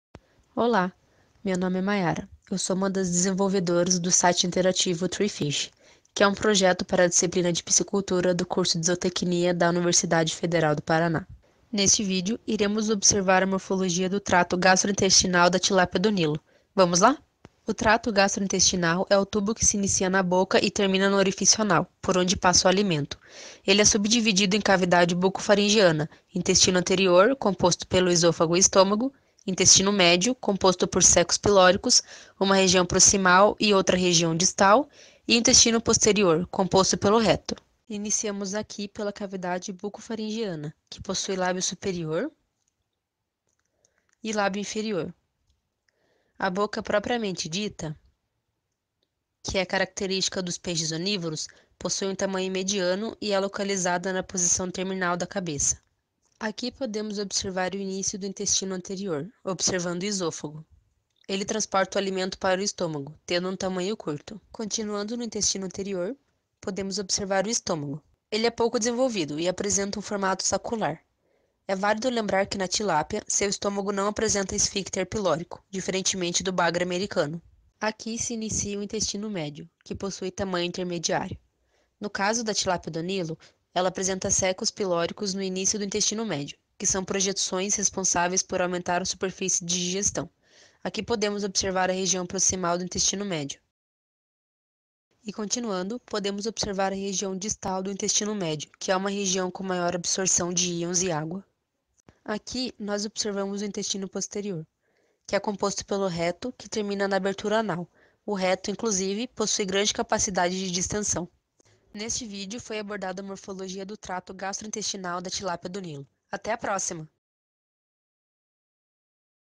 SITE INTERATIVO THREE FISH l TRATO GASTROINTESTINAL DA TILÁPIA-DO-NILO (Oreochromis niloticus)
O presente vídeo apresenta conteúdo audiovisual referente a morfologia do trato gastrointestinal da Tilápia-do-Nilo (Oreochromis niloticus).

O vídeo apresenta conteúdo de mídia autoral do Site Interativo Three Fish, que é um site desenvolvido para a disciplina de Piscicultura do curso de Zootecnia da Universidade Federal do Paraná (UFPR), pelos graduandos Agne, Denyus e Mayara.